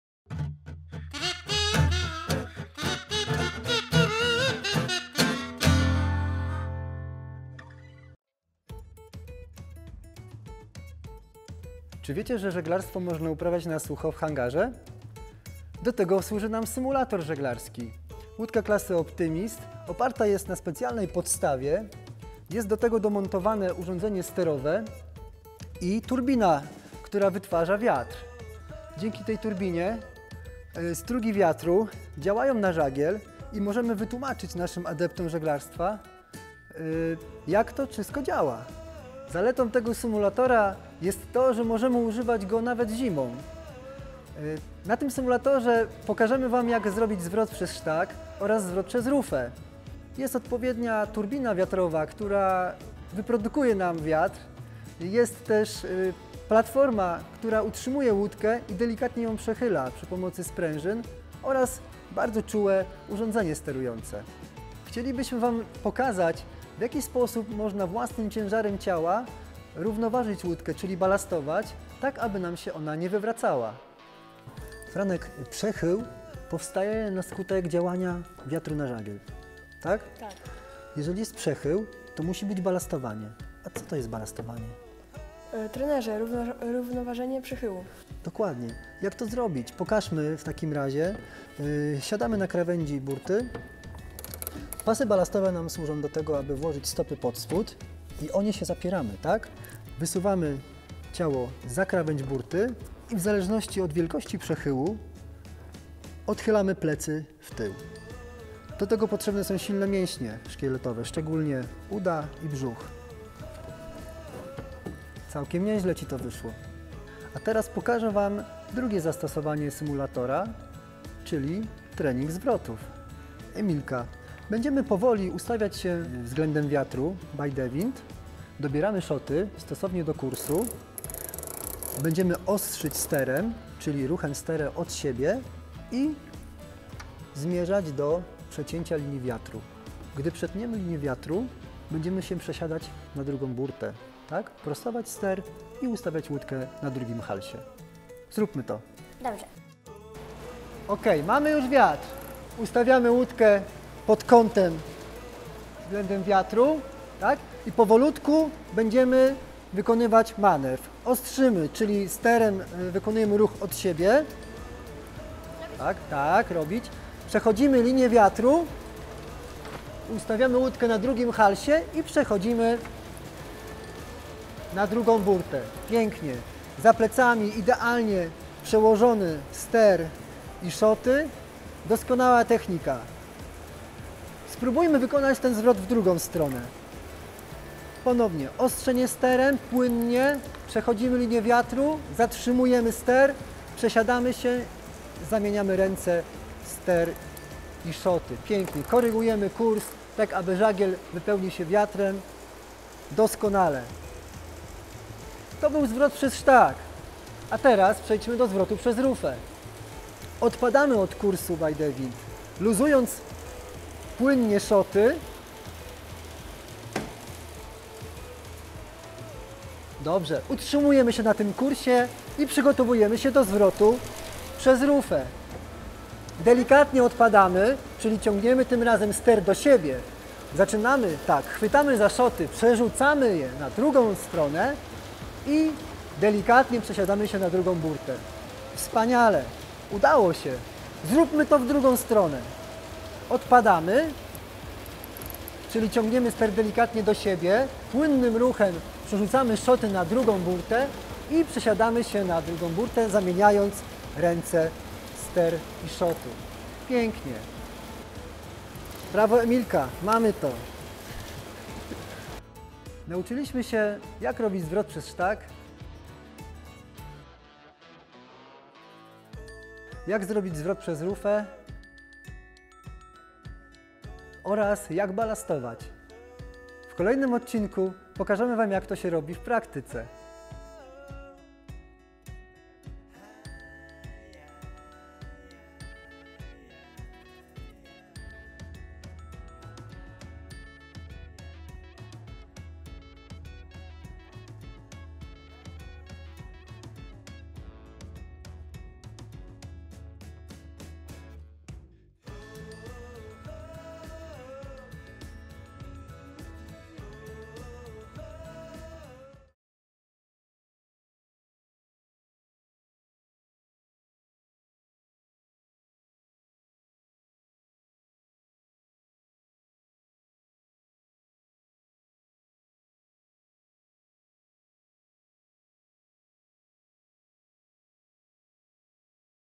POLSAILING: Wirtualna Lekcja Żeglarstwa - 5. Symulator żeglarski
Wirtualna Lekcja Żeglarstwa to nowy element Ogólnopolskiego Programu Edukacji Żeglarskiej PolSailing dedykowany szkołom i klubom żeglarskim. W tej części poznacie podstawowe manewry żeglarskie, takie jak zwrot przez rufę, zwrot przez sztag, czy trymowanie żagla, a wszystko w suchym hangarze.